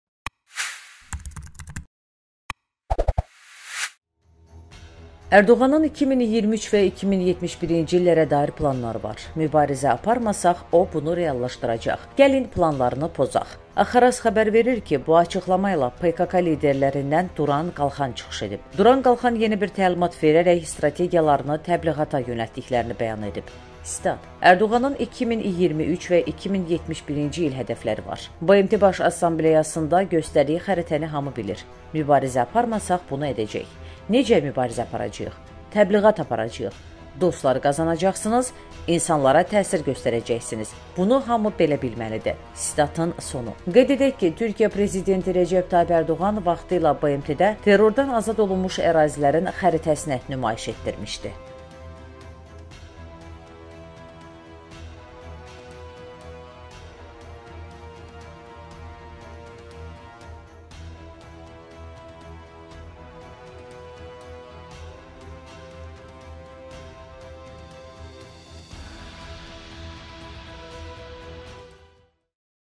QALXANDAN ŞOK TƏLİMAT: ƏRDOĞANIN PLANLARINI POZUN!
Ərdoğanın 2023 və 2071-ci ilə dair planları var. Mübarizə aparmasaq, o, bunu reallaşdıracaq. Gəlin, planlarını pozaq.